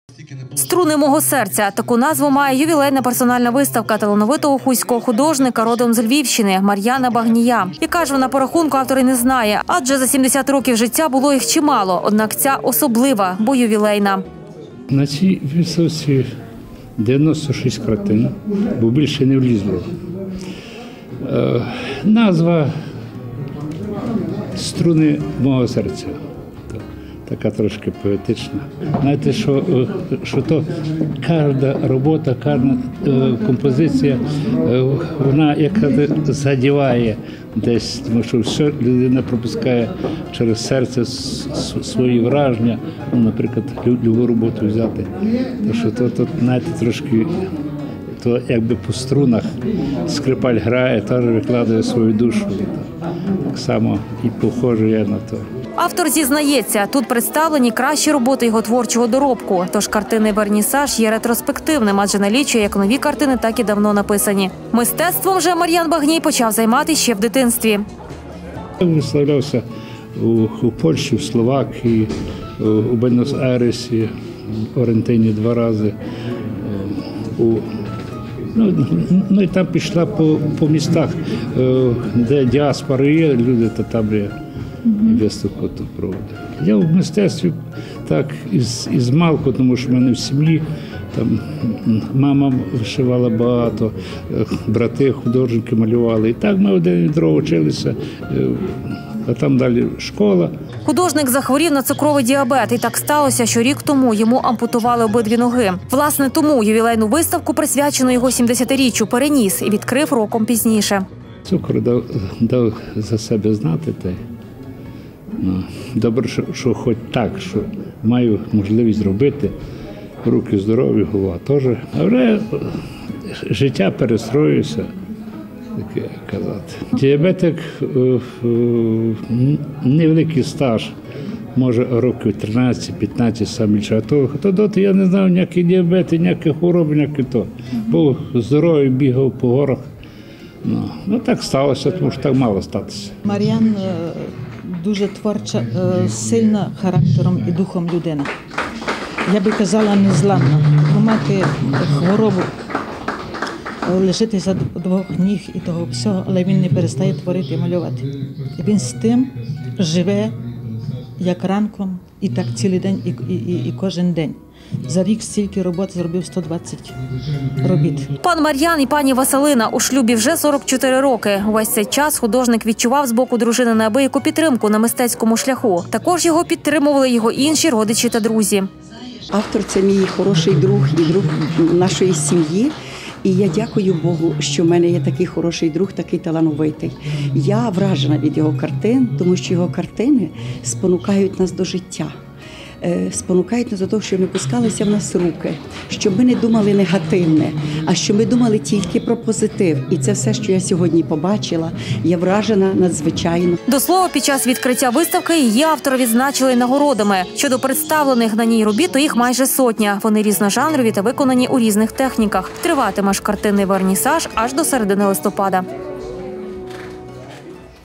«Струни мого серця»
У Хусті, у міській картинній галереї відкрили персональну виставку талановитого Мар’яна Багнія. Присвячена вона 70-річчю автора і має назву «Струни мого серця». Щодо представлених робіт, то їх майже сотня, вони різножанрові та виконані у різних техніках.